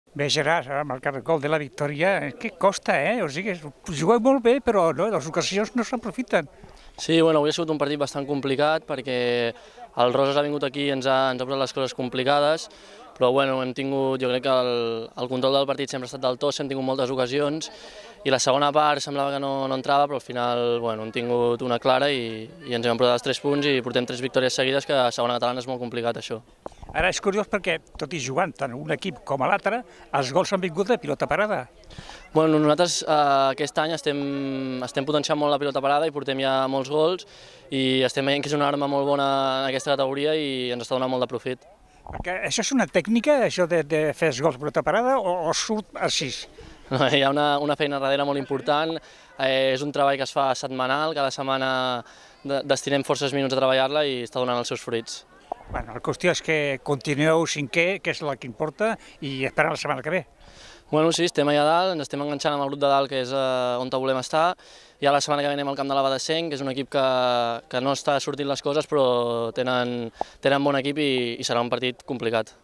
2018 11 04 2CAT1 Tossa 3 Base Roses 2 Gerard Carrillo
Gerard Carrilo, autor del gol de la victòria del Tossa, ens explica com ha vist el partit.